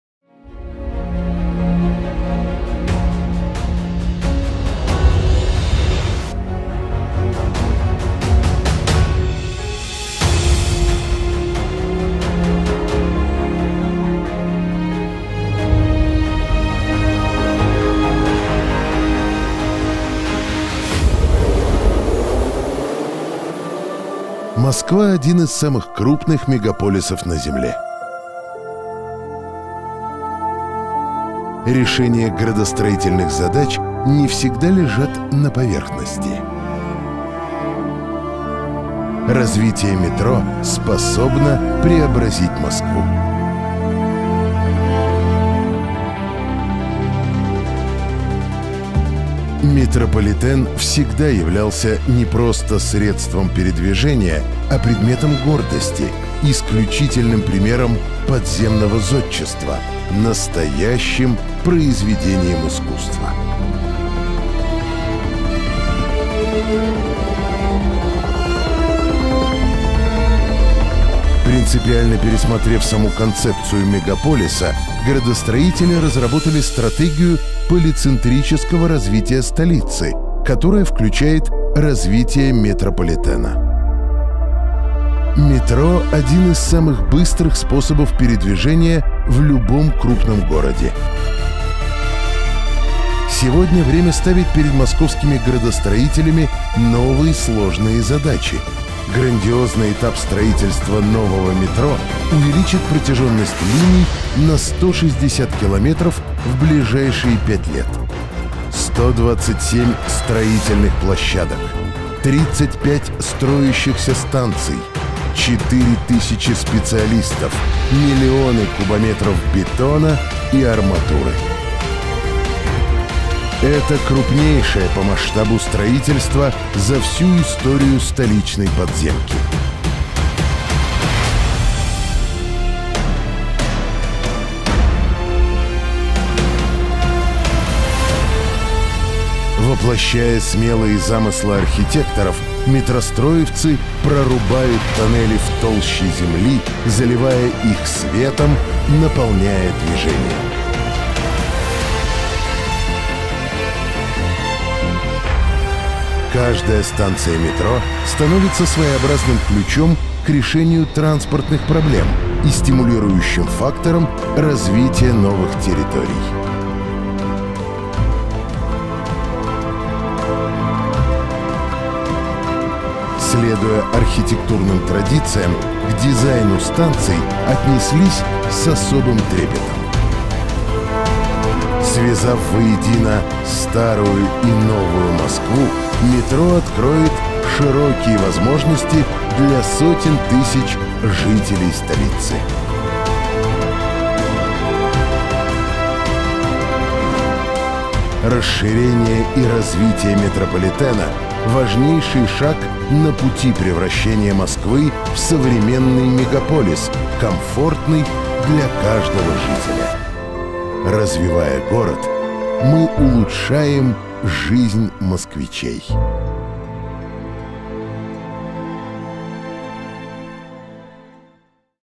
Метро вырастет на 78 станций и 160 км тоннелей
С 2011 по 2020 год в Москве планируется построить более 160 км линий метро и 78 новых станций, 14 из них уже открыты. В результате через пять лет 93% наших жителей получат метро в шаговой доступности. Сейчас этот показатель составляет 80 %. Сегодня работы ведутся на 35 станциях метро и 6 электродепо. В настоящее время на строительстве метрополитена в Москве задействовано более 35 тысяч человек. Перегонные тоннели прокладывают 22 тоннелепроходческих щита.